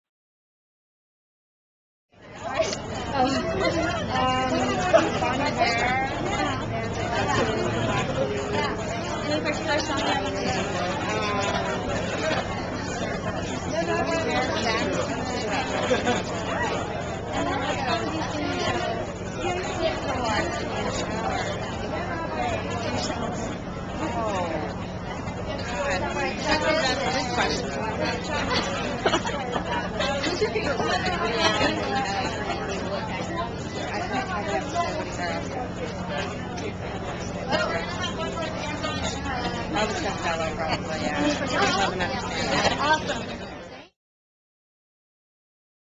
Red Carpet Interview with Mary Louise Parker
Created on November 23, 2009 using FlipShare. PCM's red carpet interview with actress Mary Louise Parker at the 2009 MTVu Woodie Awards